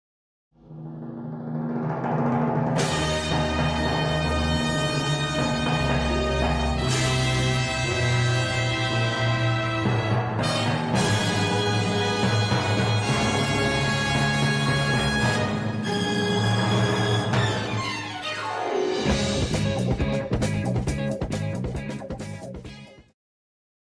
Space: 1999 25-foot Eagle 1 model reveal
A scene from my Open House arrangement on June 29th 2024.